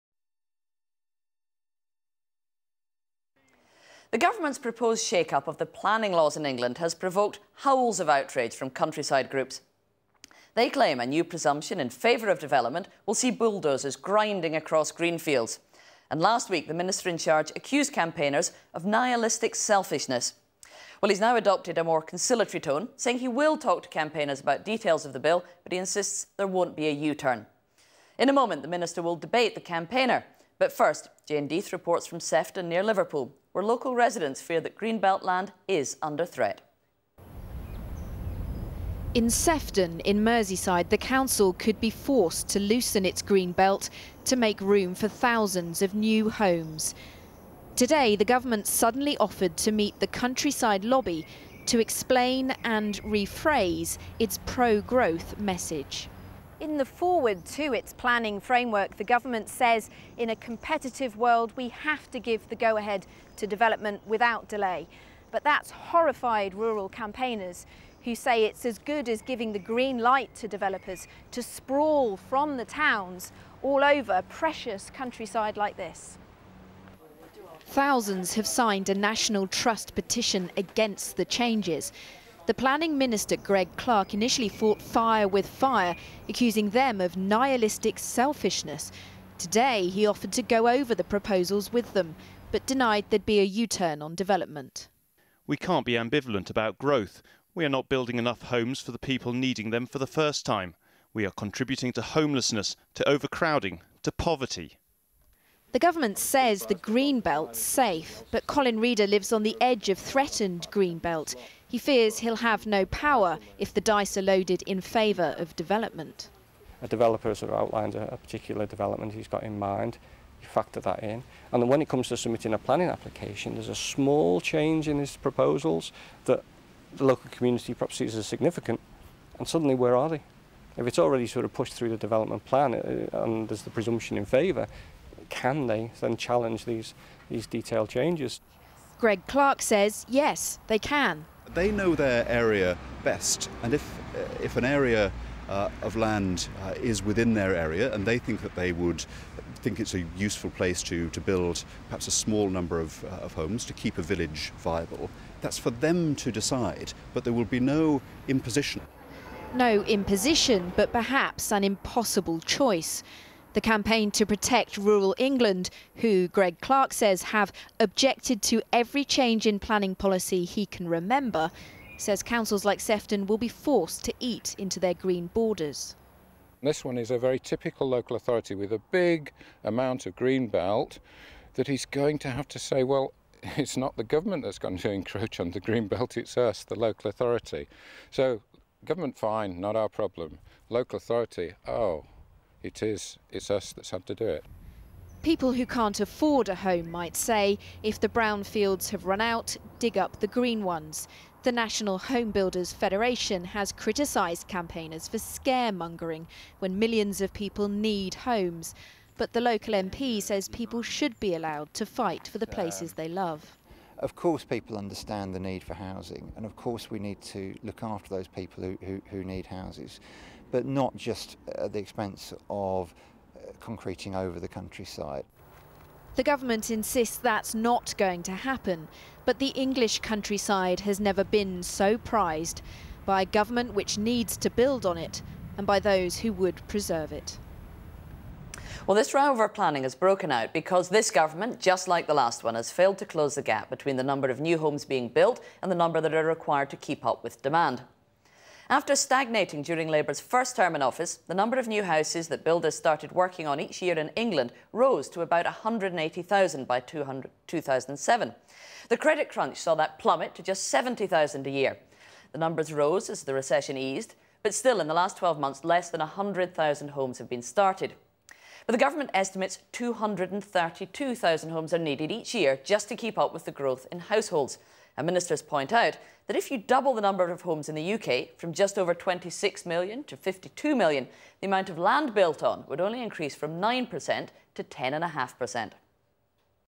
Planning shake-up prompts outrage
Controversial planning changes could mean more than 1,000 extra "major developments" being approved every year in England - but critics fear they could end up "concreting the countryside".